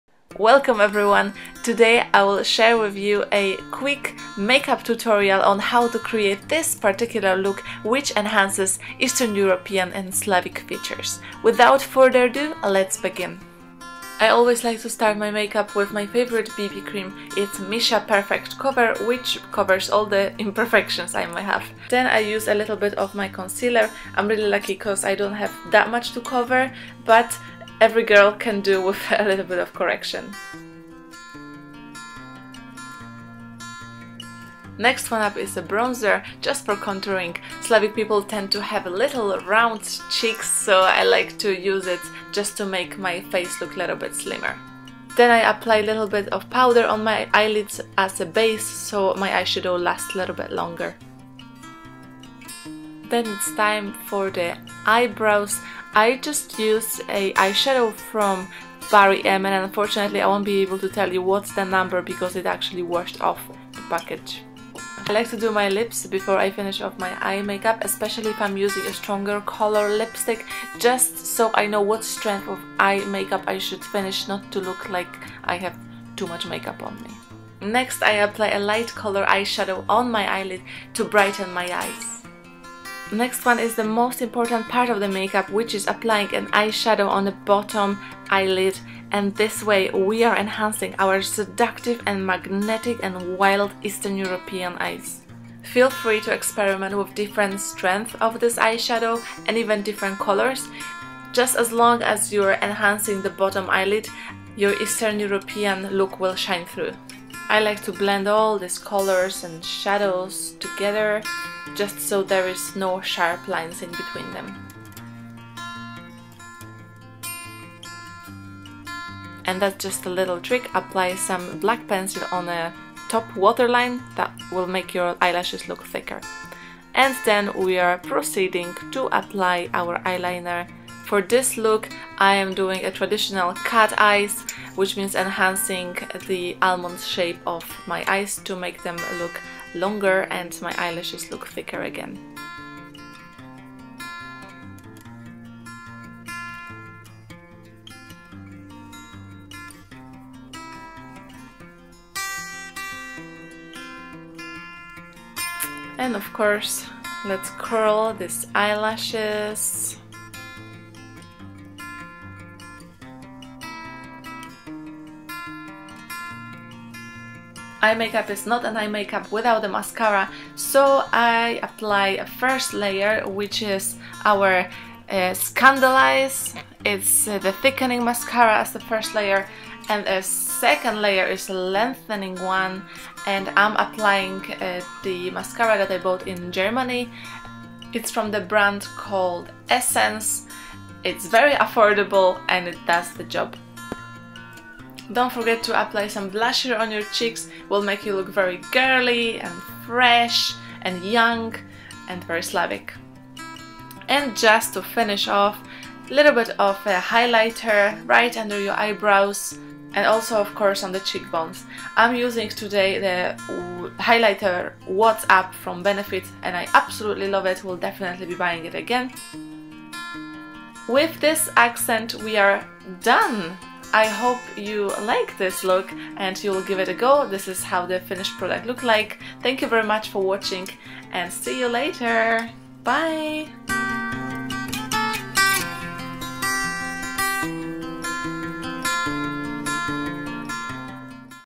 Makeup Enhancing Eastern European (Slavic) Features //Makeup Tutorial - Benefit - Mac - L'oreal
Regardless the title, this makeup is not only for Eastern Europeans! I invite you to try something new and play with your look whatever part of the world you come from :) Enjoy!

The cosmetics used:

1) BB cream - Missha Perfect Cover
2) Concealer - Inexpensive Concealer from Boots, I cannot remember exactly, unfortunately all the labels have washed off… I can only see the description "Fair 1" on the top. It looks similar to "Natural Collection Cover-up Cream”
3) Bourjois Paris Maxi Delight Bronzer
4) Snow White BB Powder
5) Eyebrows - Barry M Dazzle Dust (the number has washed off but it's a dark bronze colour)
6) Lip Pencil "Dervish" from Mac
7) Essence XXXL Shine Lip Gloss - "Nude Candy"
8) Eye Shadow - Barry M Dazzle Dust "Athena"
9) L'Oreal Shimmer Eye Shadow, Chrome Shine Beige Number 168 -Brun Lame
10) Waterproof Eyeliner Pen from Essence
11) Scandaleyes Mascara - Very Black
12) I Love Extreme Volume Mascara from Essence
13) Rimmel Powder Blusher (the number has washed off :( )
14) Watt's up Face Highlighter from Benefit